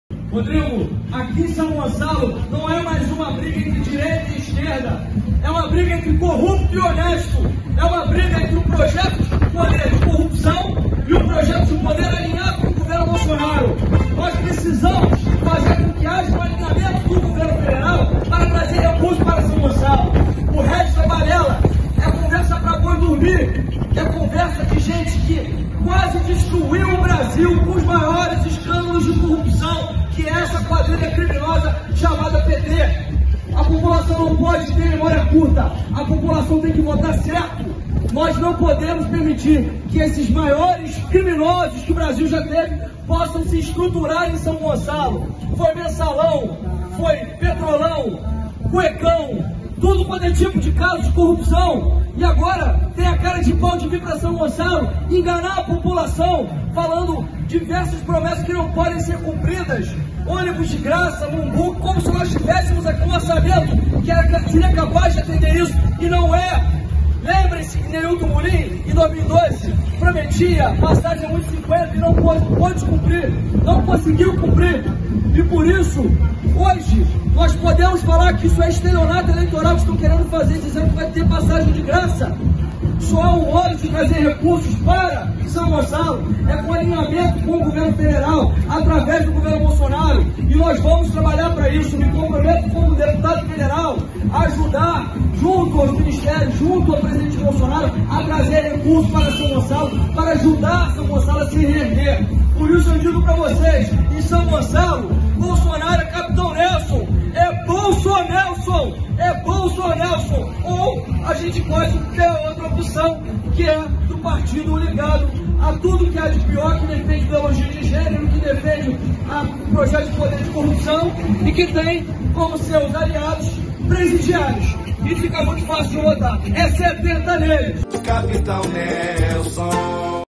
SÃO GONÇALO NÃO PODE SER TOMADO PELO PARTIDO QUE QUASE DESTRUIU O BRASIL
Em São Gonçalo, não é mais uma disputa entre esquerda e direita, é entre um projeto de poder de corrupção e um projeto alinhado com o Governo Bolsonaro. Vamos de 70 para impedir que a facção criminosa chamada PT possa destruir SG.